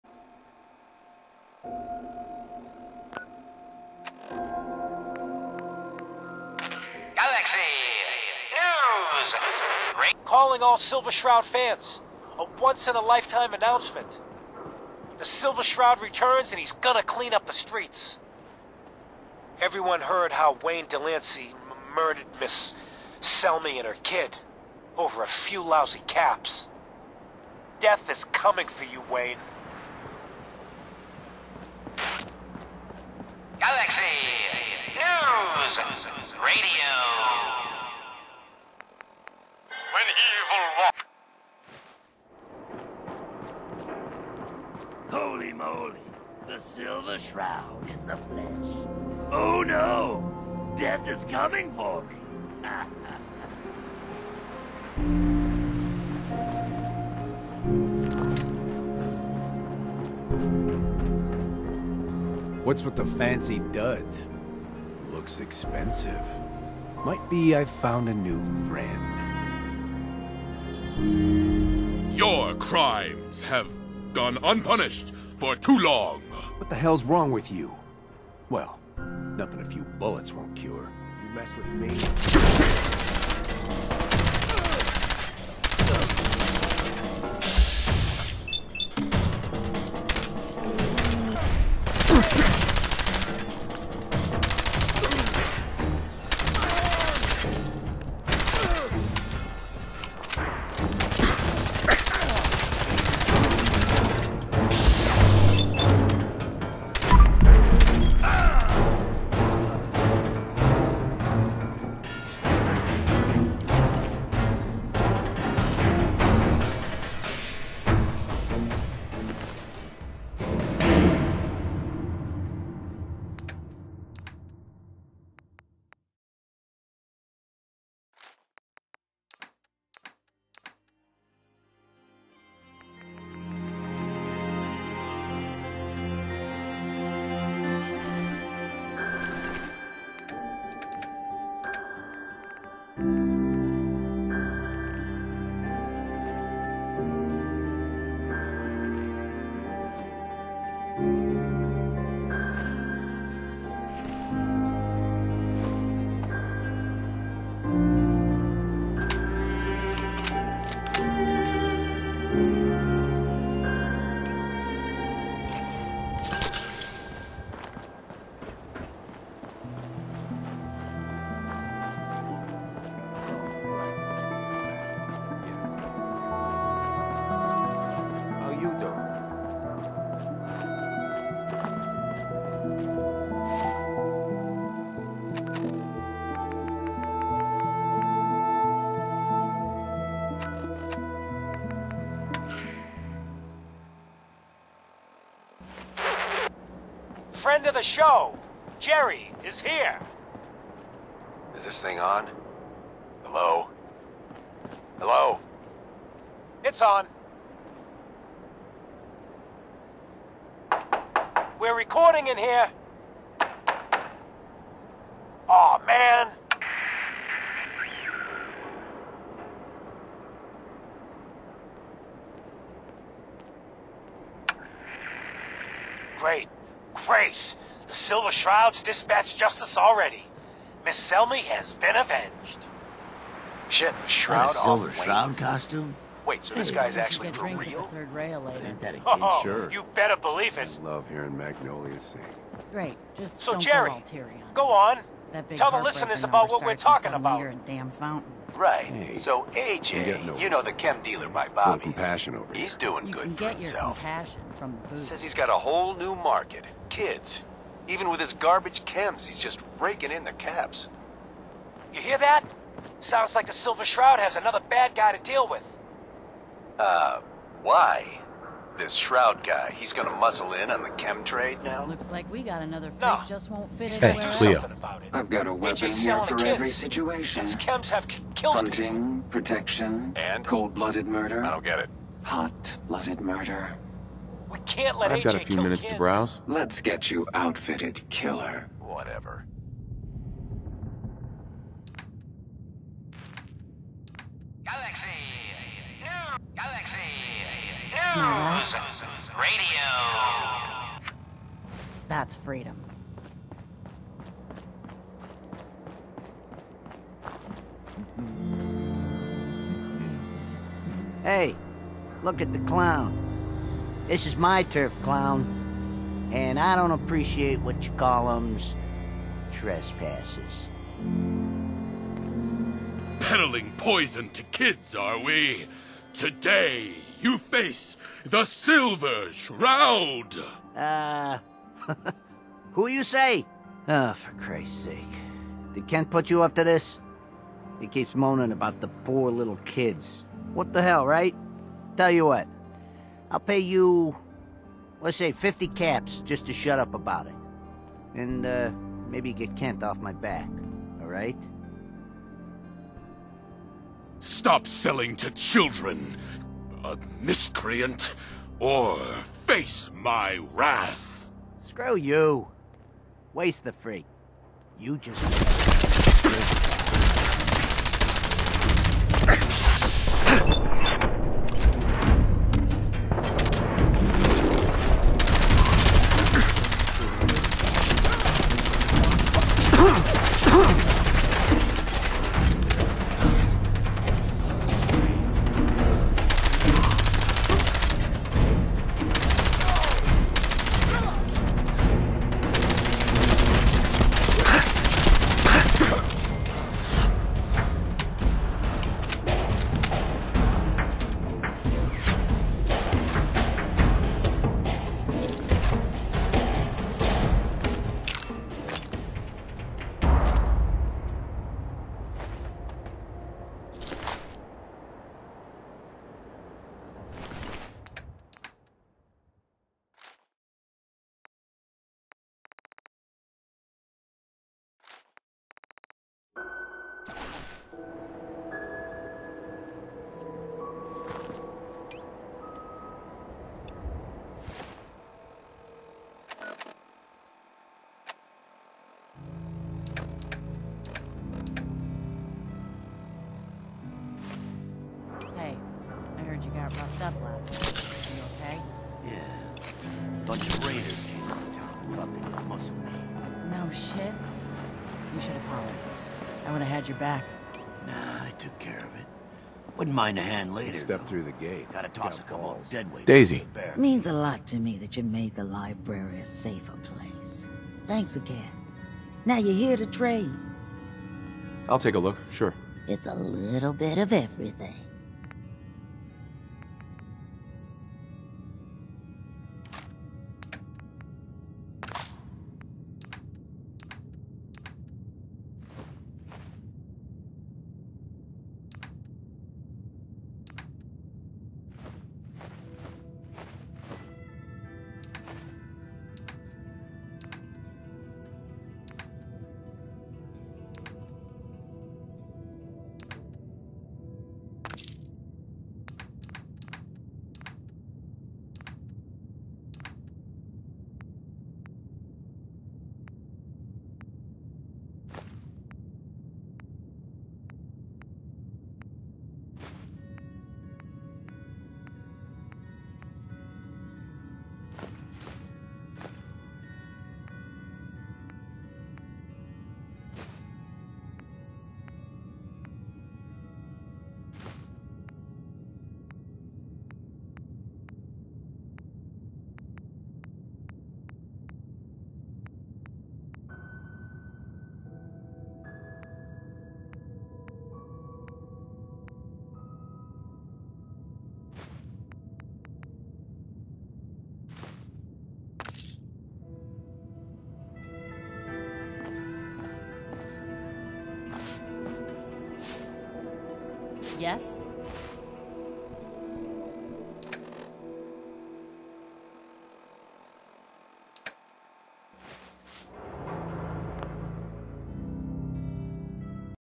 The Silver Shroud Part 1
The first part in my series, The Silver Shroud. Join me as I take on the identity of The Silver Shroud, a classic comic book vigilante. In this episode I take on the merciless killer, Wayne Delancy and the peddler of poison, AJ.